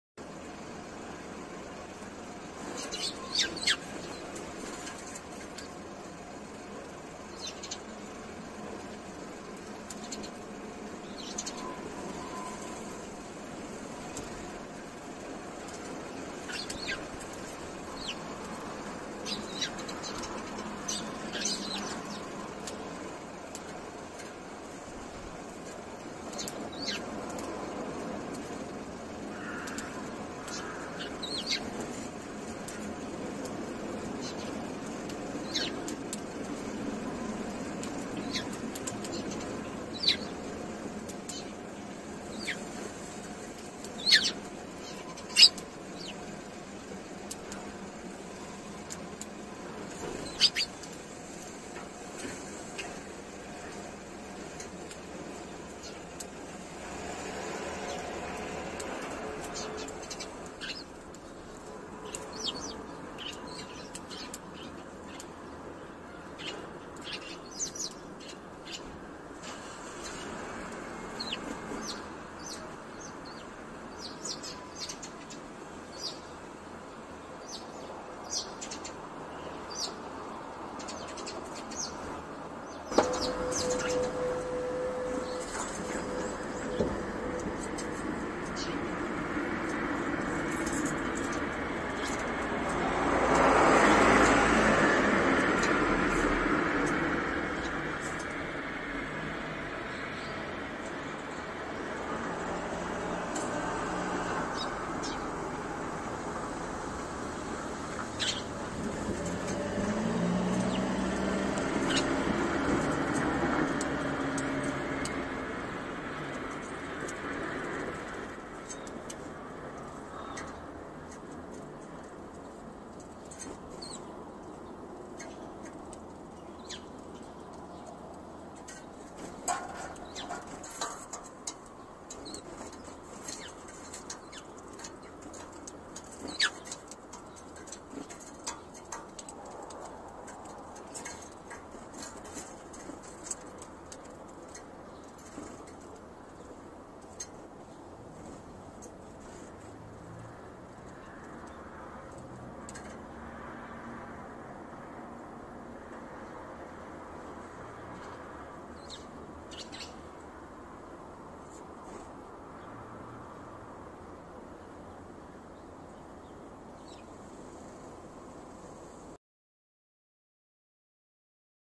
スズメの朝食
2022年2月5日

米粒をバラまいておくと食べに来ます。

朝と寝床へ帰るまでの夕方ラストは結構来てくれます☺️

もれなく💩が付いてくるのでお掃除もかかせませんが😅😂